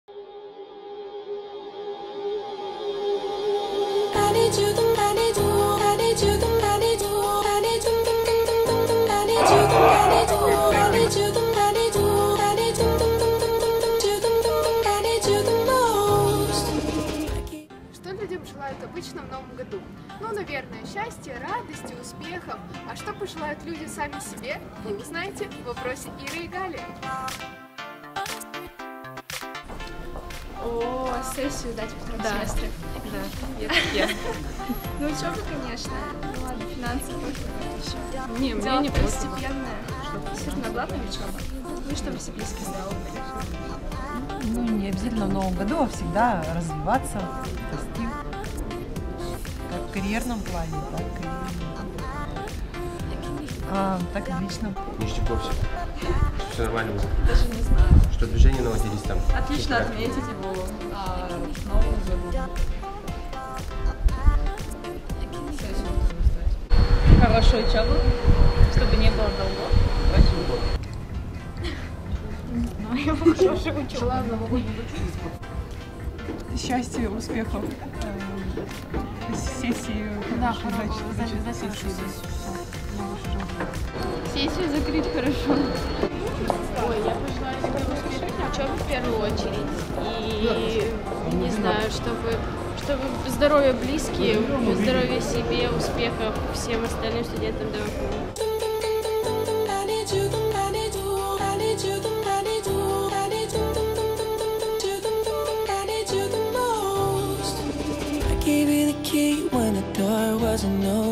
Что вы пожелаете себе в Новом году?
ShotNews  опрашивает студентов ДВФУ об их новогодних желаниях.